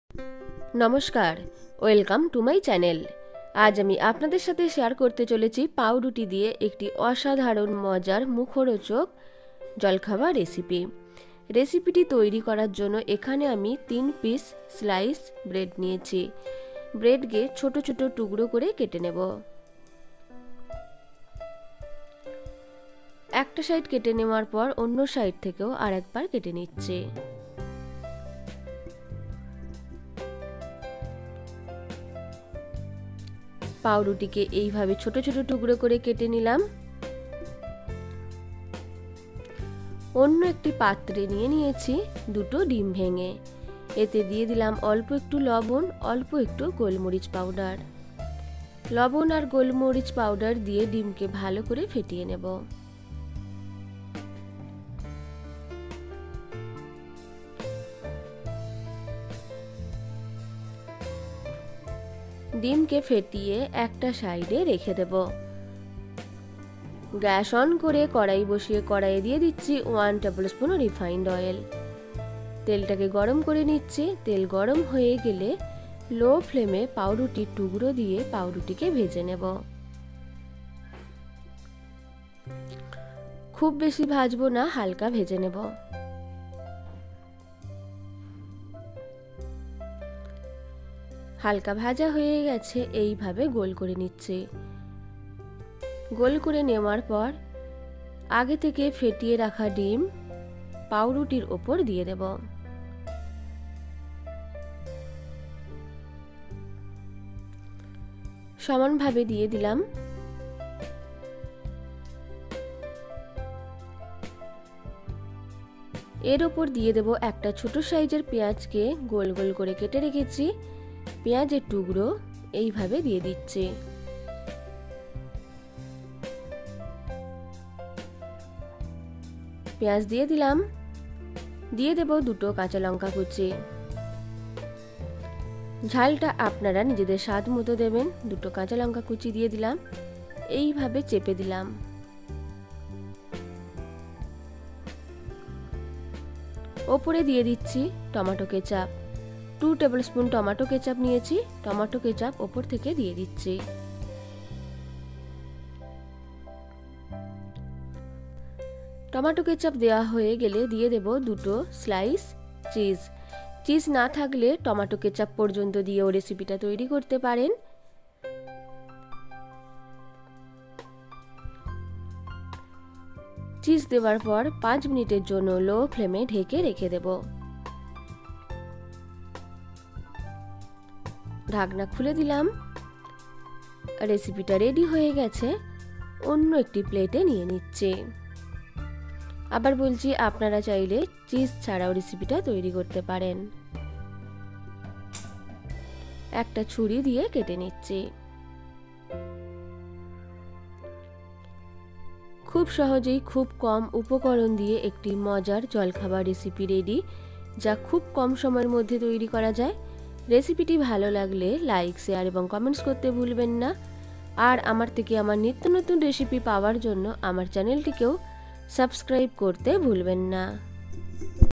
পাউরুটি দিয়ে একটি মজার মুখরোচক নাস্তা রেসিপি যা একবার খেলে বারবার খাবেন।
Ingredients

3 pieces slice bread
 2 pieces egg
1 piece small size onion
2 pieces chopped green chilli
2 tbsp tomato sauce
2 pieces slice cheese ( optional  )
salt
1 tbsp refined oil
little black pepper powder